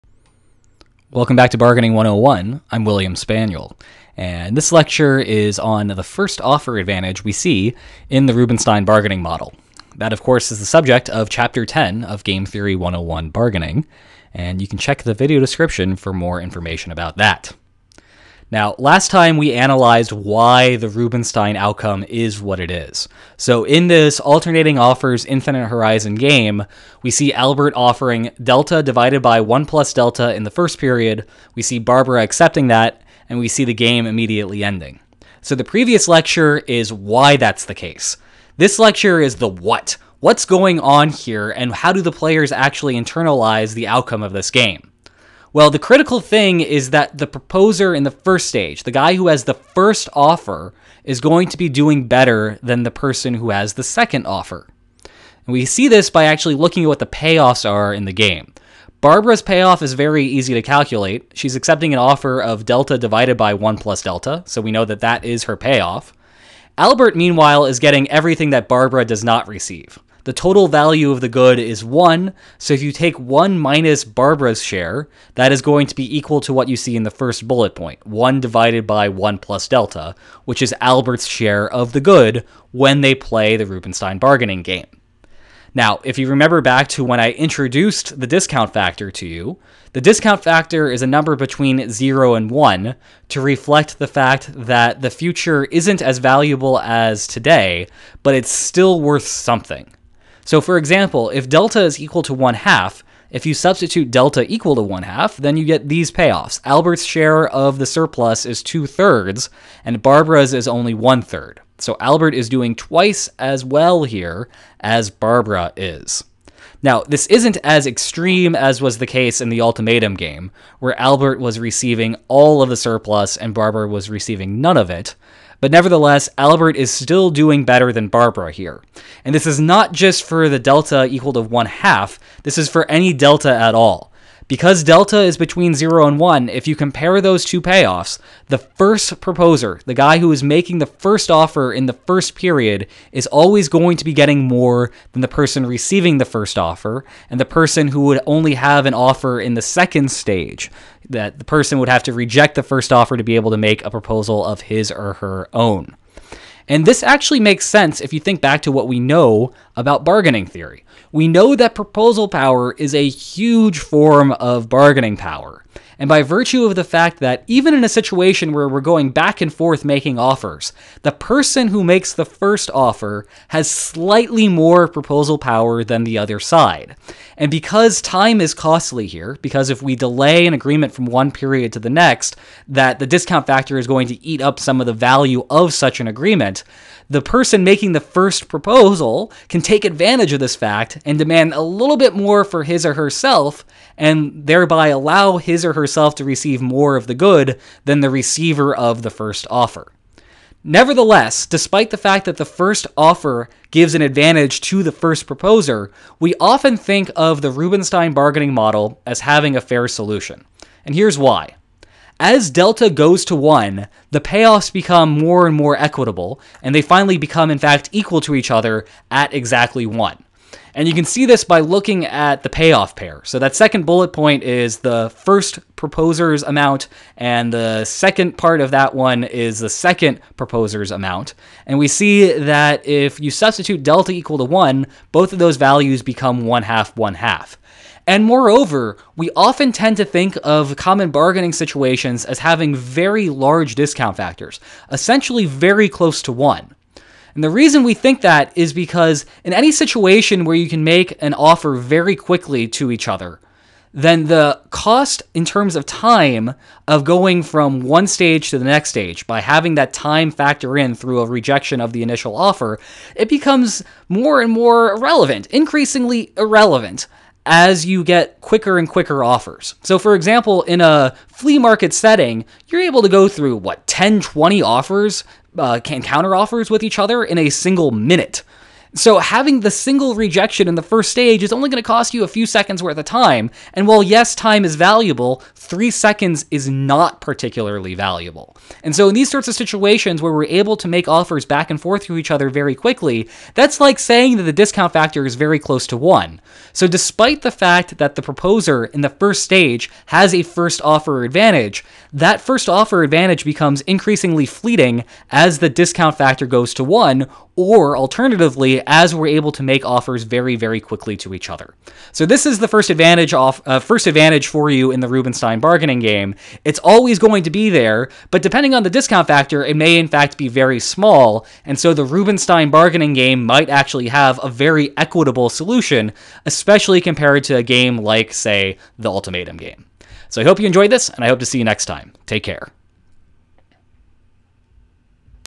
Bargaining 101 (#13): First Offer Advantage
Game Theory 101: Bargaining

The last lecture explained how to get to the solution of Rubinstein bargaining. This lecture analyzes the welfare implications of that solution. We find that the person who makes the first offer still has an advantage, though it can be fleeting depending on the discount factor.